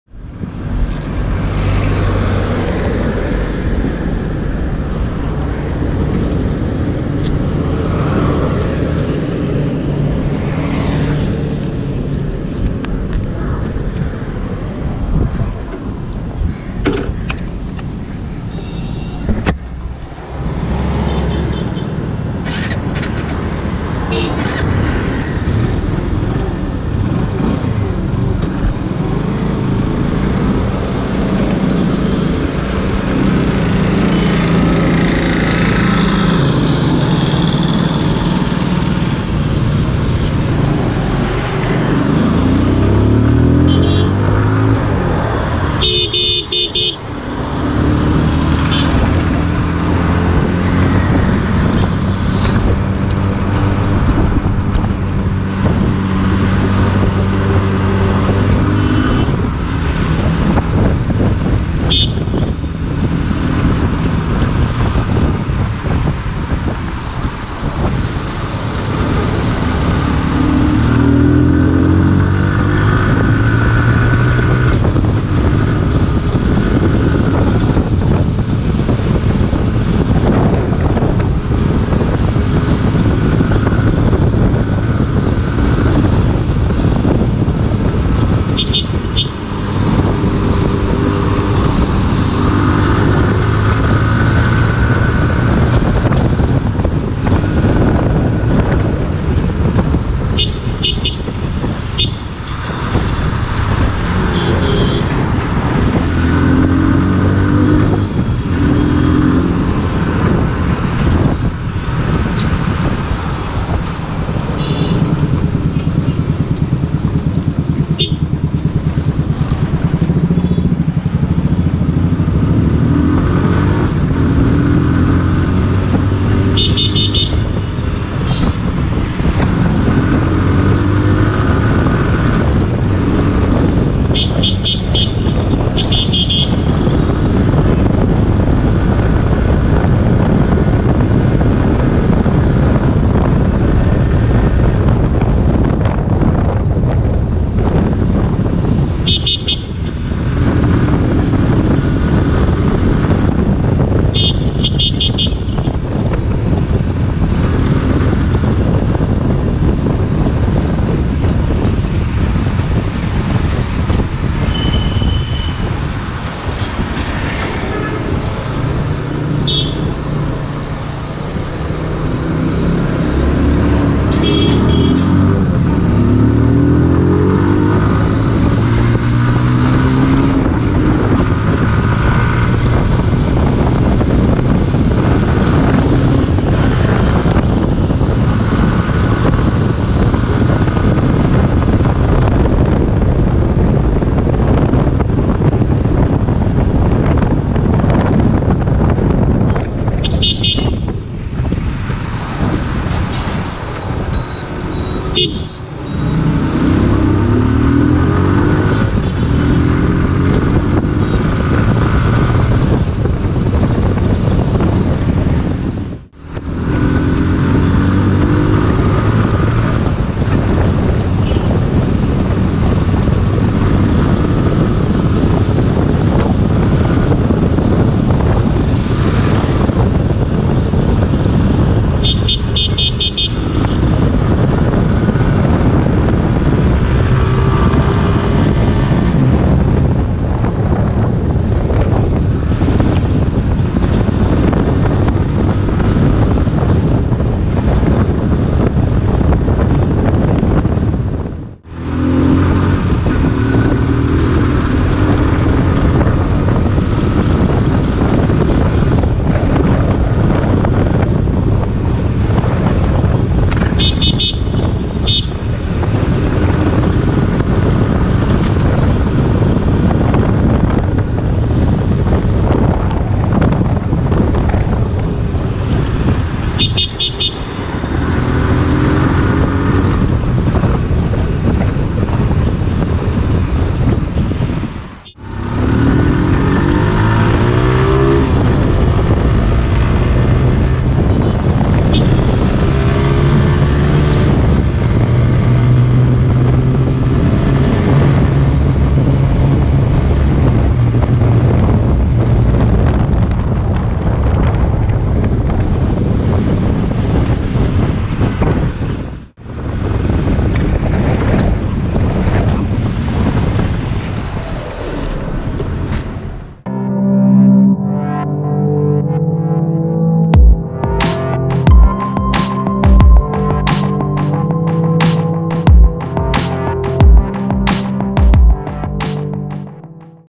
Apache RTR 160 Race Edition Street Ride Raw Footage Fast Sporty Fun
No talk, no music, pure motorcycling fun. Here's the raw footage video of me riding the new TVS Apache RTR 160 Race Edition through the streets.
First Ride Review coming this week.
Though the RTR 160 4V is like a new benchmark in the segment, the RTR 160 Race Edition still has the intensity, the raw brute force, and loads of fun.